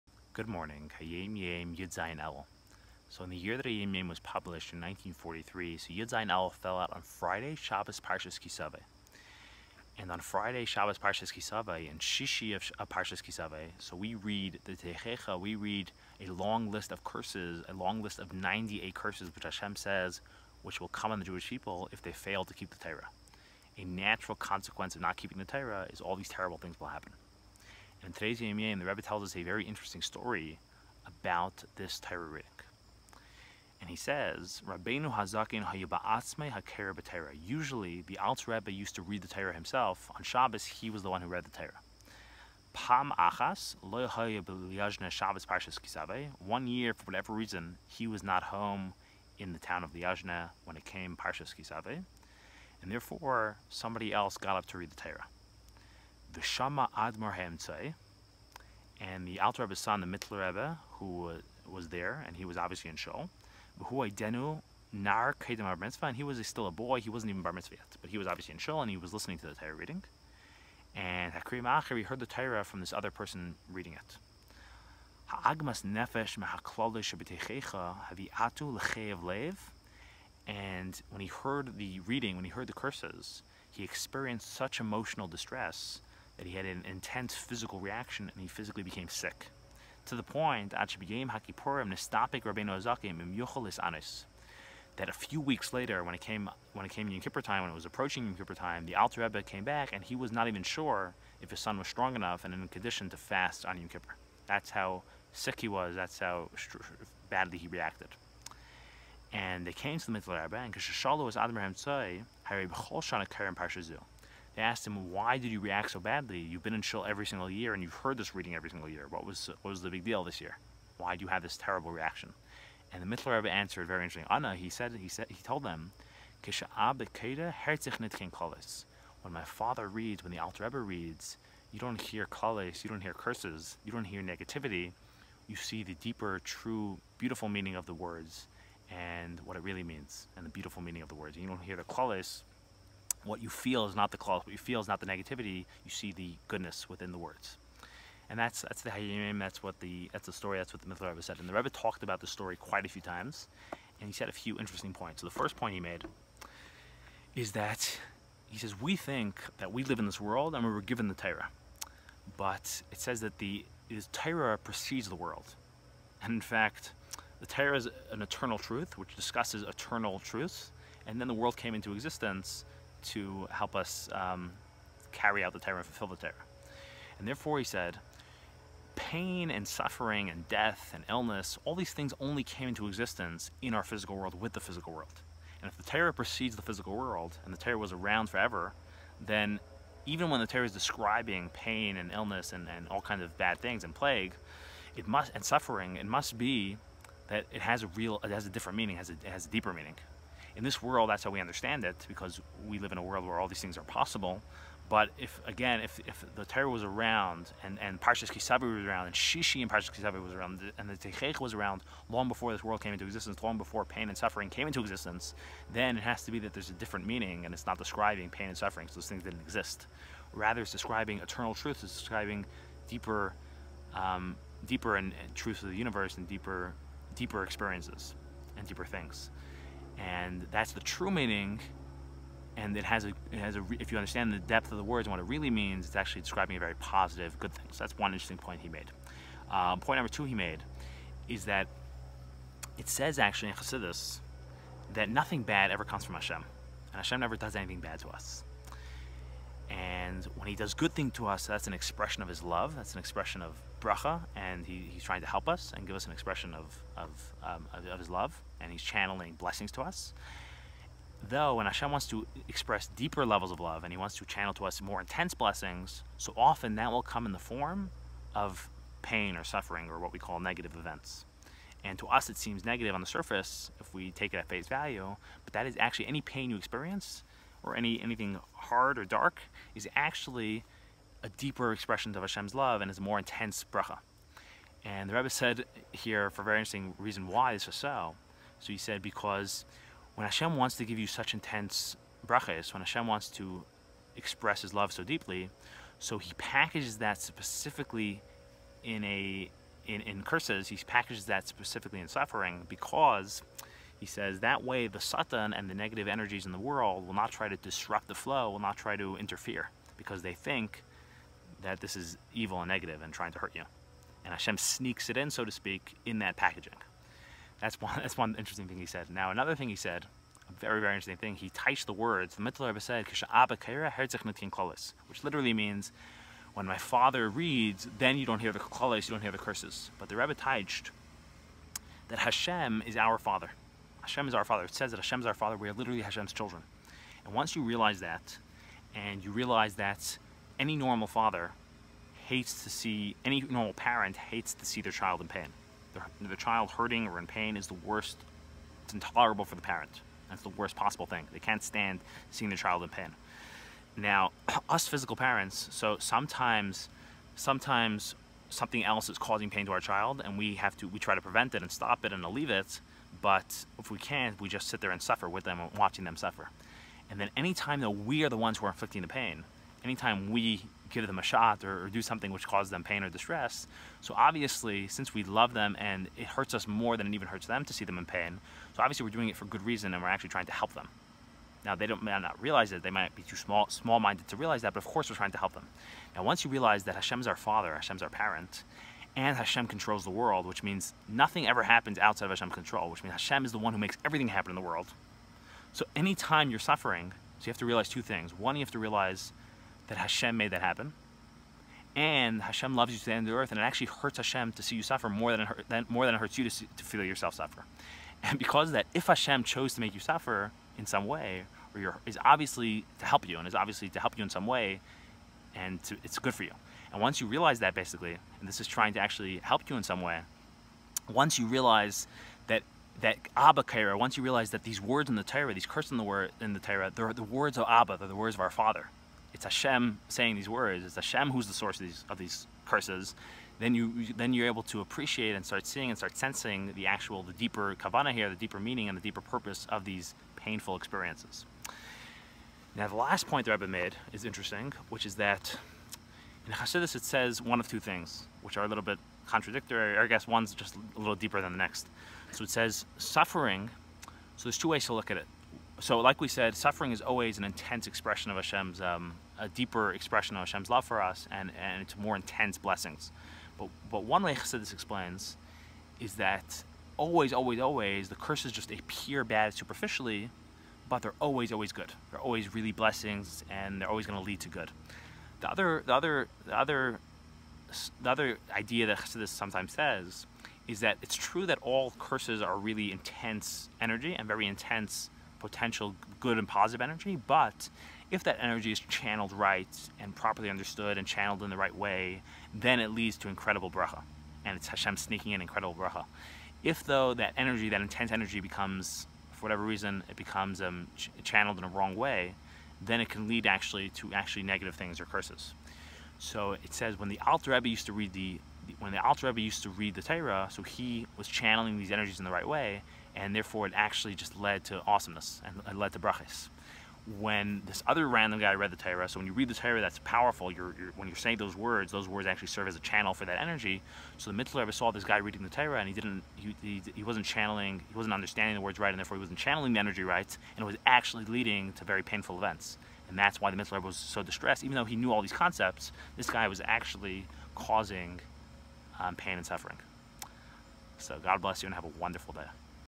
Hayom Yom Yud Zayin Elul: THE TRUE MEANING OF THE CURSES IN THE TORAH!
Hayom Yom of the Rebbe, taught by Yanky Krasnianski.
17 Elul: The curses in the Torah are actually blessings when understood properly.
00:03 - 00:33. Intro.
00:34 - 02:30. The story with the Mitiler Rebbe.
02:31 - 03:58. Addition #1 from the Rebbe: Why the curses in the Torah must have a positive meaning.
03:59 - 05:18. Addition #2 from the Rebbe: More intense blessing are packaged as curses so that evil won't object and interfere.
05:19 - 07:20. Addition #3 from the Rebbe: You will begin to see the true purpose behind the "curses" when you remember that our father (Hashem) is the source of them.
07:21 - 08:59. Addition #4 from the Rebbe: Why the Mitiler Rebbe was distressed despite knowing that the curses are really positive.

RELEVANT SOURCES:
1)

HAPPY BIRTHDAY:
1.